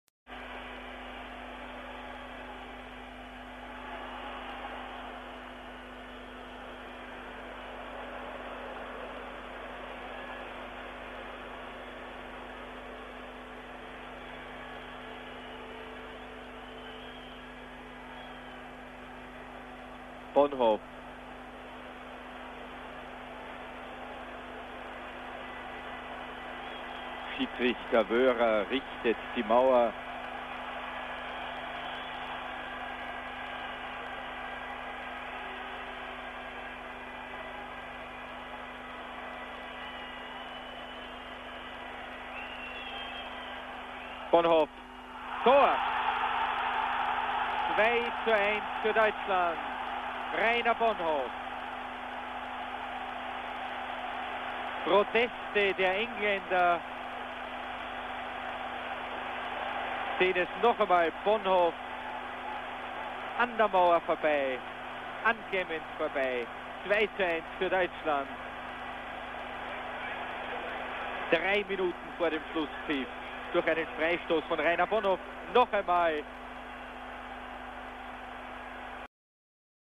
Rainer Bonhof vs Ray Clemence ... Part 1
22 February 1978, Munich, Olympic stadium, Germany vs England friendly ... 86th minute.